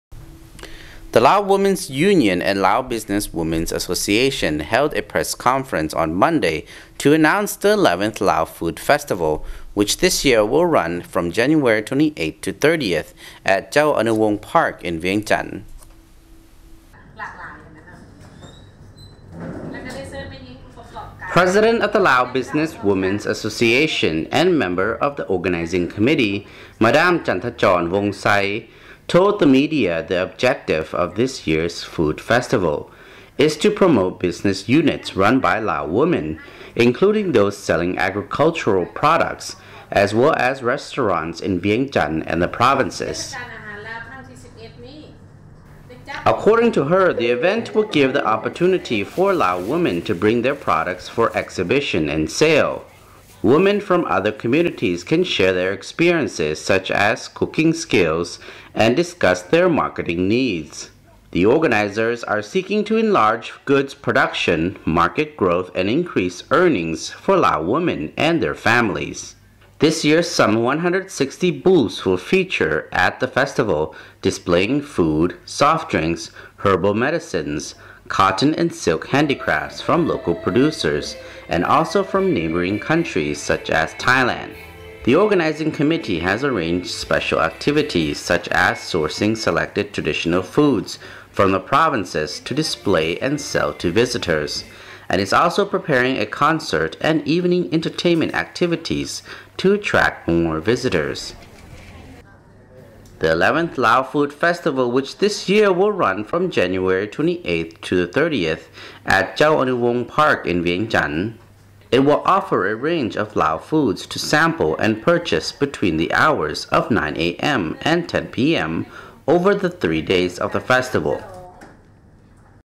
Lao NEWS on LNTV: 11th Lao Food Festival will run from January 28-30.12/1/2016
VO 11th Lao Food Festival will run from January 28-30 at Chao Anouvong Park in Vientiane
INTRO: The Lao Women's Union and Lao Business Women's Association held a press conference on Monday to announce the 11th Lao Food Festival, which this year will run from January 28-30 at Chao Anouvong Park in Vientiane.
STORY: President of the Lao Business Women's Association and member of the organising committee, Madame Chanthachone Vongxay, told the media the objective of this year's food festival is to promote business units run by Lao women including those selling agricultural products as well as restaurants in Vientiane and the provinces.
According to her, the event will give the opportunity for Lao women to bring their products for exhibition and sale. Women from other communities can share their experiences such as cooking skills and discuss their marketing needs. The organisers are seeking to enlarge goods production, market growth and increase earnings for Lao women and their families.
This year, some 160 booths will feature at the festival, displaying food, soft drinks, herbal medicines and cotton and silk handicrafts from local producers and also from neighbouring counties such as Thailand.
The organising committee has arranged special activities, such as sourcing selected traditional foods from the provinces to display and sell to visitors and is also preparing a concert and evening entertainment activities to attract more visitors.
The 11th Lao Food Festival, which this year will run from January 28-30 at Chao Anouvong Park in Vientiane. It will offer a range of Lao foods to sample and purchase between the hours of 9am and 10pm over the three days of the festival.